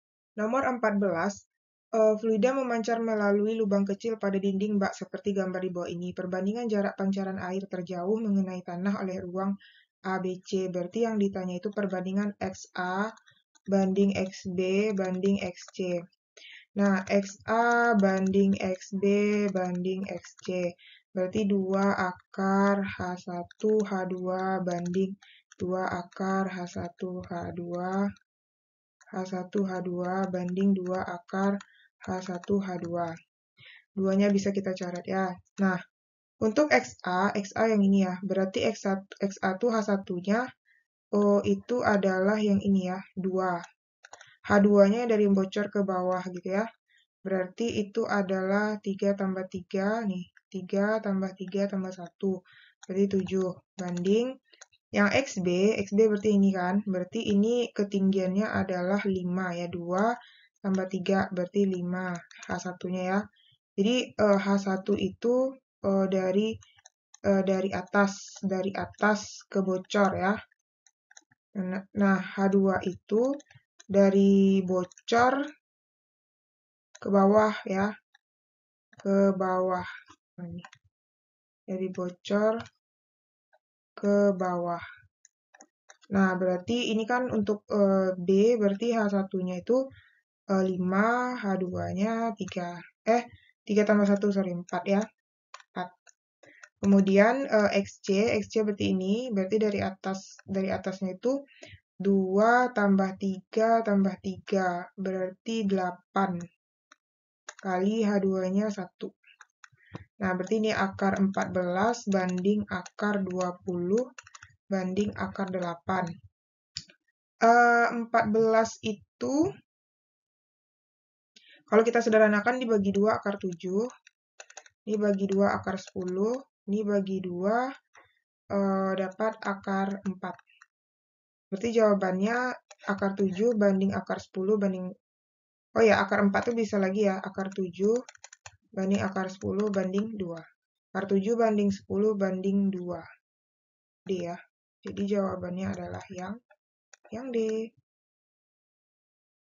ASSESMEN HARIAN no 14 kelas 11 FLUIDA intan pariwara 2023 fisika
fluida memancar melalui lbang kecil pada dinding baik seperti gambar di bawah ini. perbandingan jarak pancaran air terjatuh mengenai tanah oleh lubang A, B dan C adalah